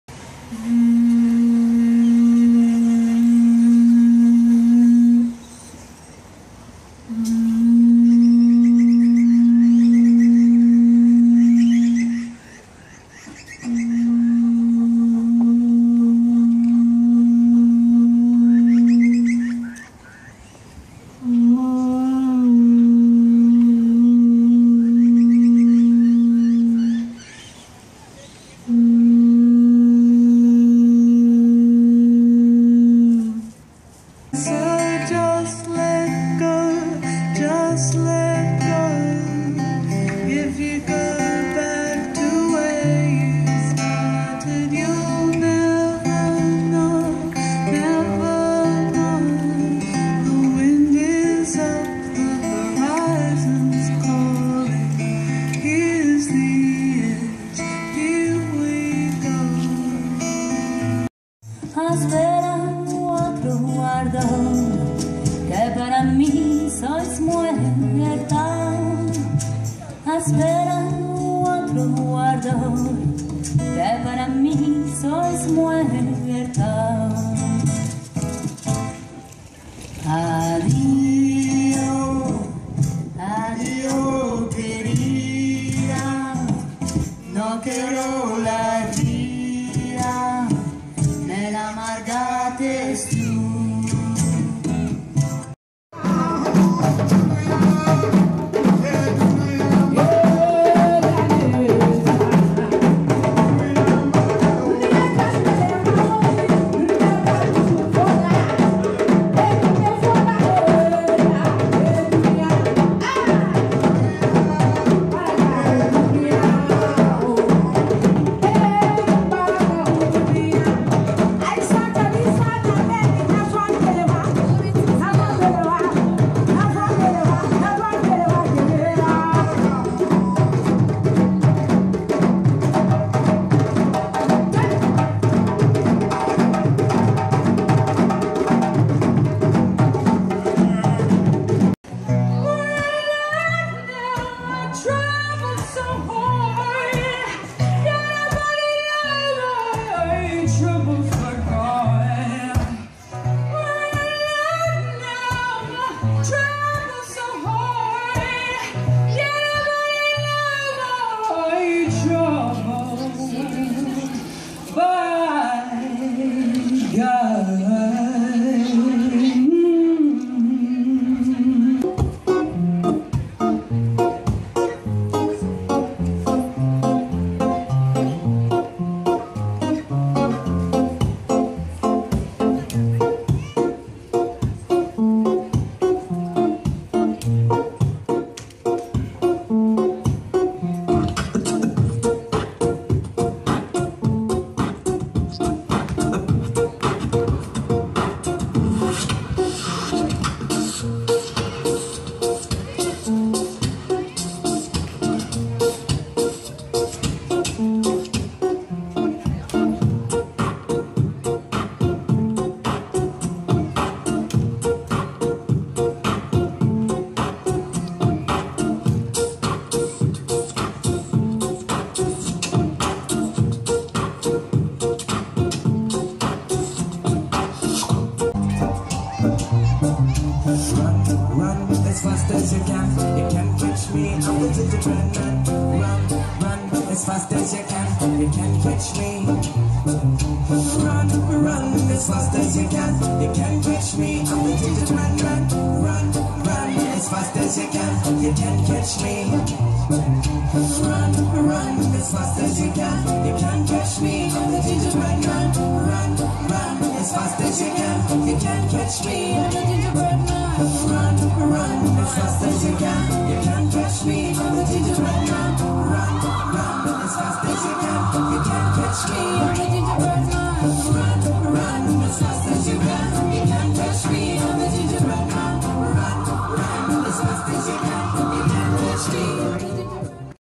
Carlton Harmony Day Festival 21 March 2015
Performers in order of appearance:
- Netani Coriakula (Fijian call for a gathering)
- Eva Popov (local artist)
- Oriente (fusion band - local artists)
- DunDunKan - local African drummers lead by Amadou Kalissa
- Katey Brooks (all the way from the UK)
- Reuben Stone (professional busker - local artist)
- Missing artist video - Seble - local traditional Ethiopian dancer
Festival was held alongside the Carlton Farmers Market held every second Saturday at the Carlton Primary School - festival organisers - Carlton Local Agencies Network & Global Echo Consultants.